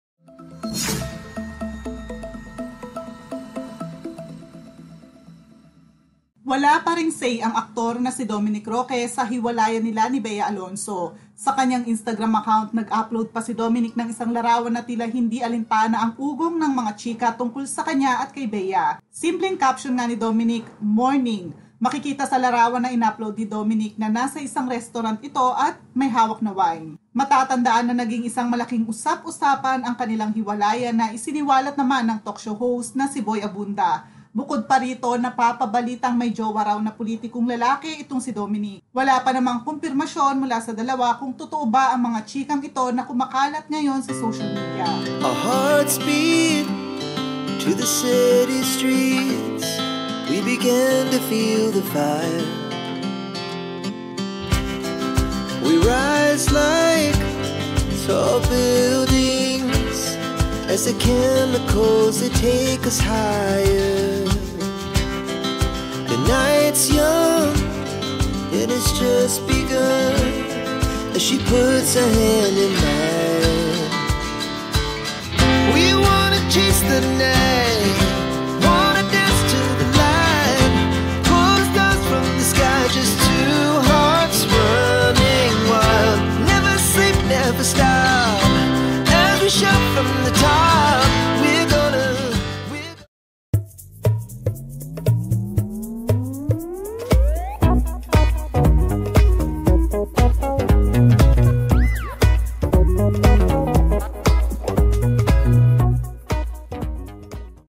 TIKOM-BIBIG pa rin si DOMINIC ROQUE Tungkol sa Gay, Condo at Gasoline Station Issue!
Dominic Roque tikom-bibig pa rin sa lahat ng mga intrigang ibinabato sa kanila ni Bea Alonzo.

» Video Editor: Camtasia Studio
» Thumbnail: Photoshop cs3